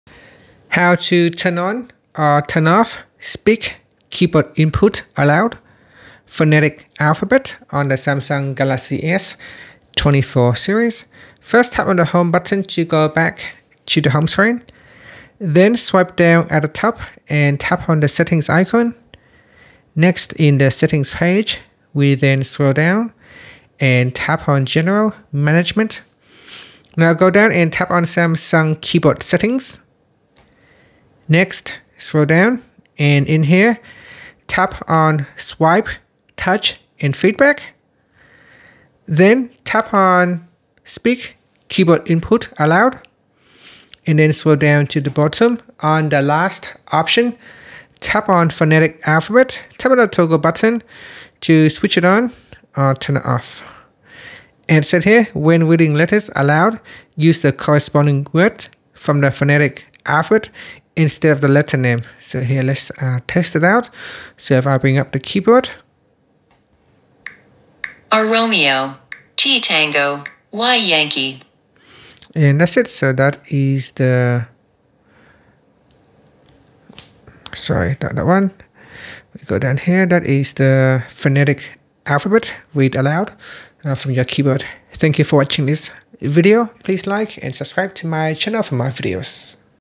Galaxy S24/S24+/Ultra: How to Turn On/Off Speak Keyboard Input Aloud Phonetic Alphabet
Welcome to our comprehensive guide on activating or deactivating the Speak Keyboard Input Aloud Phonetic Alphabet feature on your Galaxy S24, S24+, or Ultra! In this step-by-step tutorial, we'll walk you through the process, making it easy for you to manage this innovative function. Whether you're new to your device or seeking to customize your user experience, we've got you covered. Watch now and master your Galaxy's settings effortlessly!